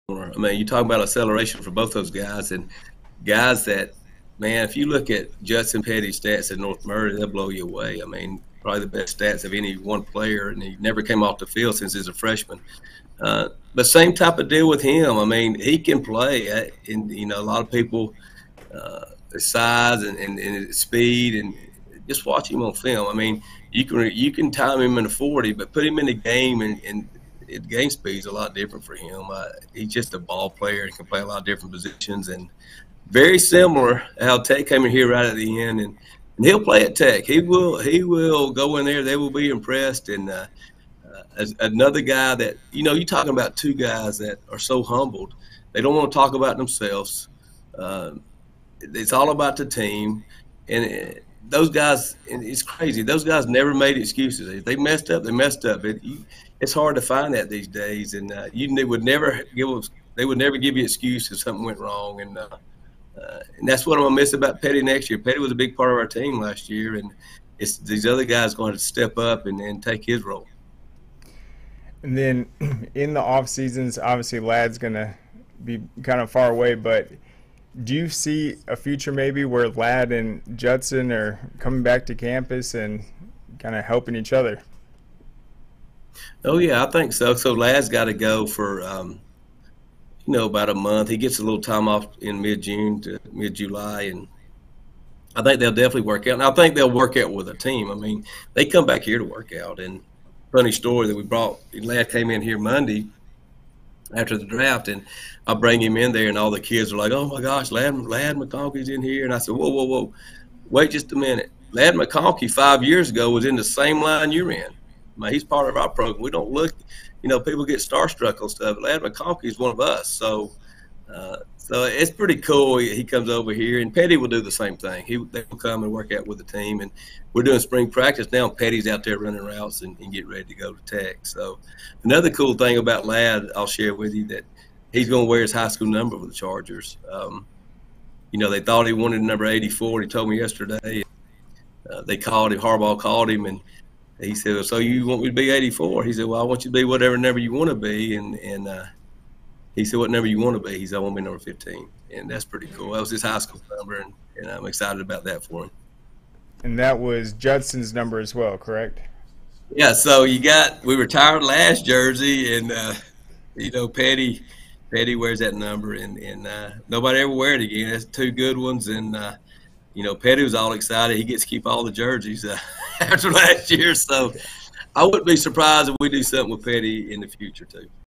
Comparing the Similarities Between Ladd McConkey and Judson Petty
Coach Poag talks the similarities and differences between his star receivers that came through the program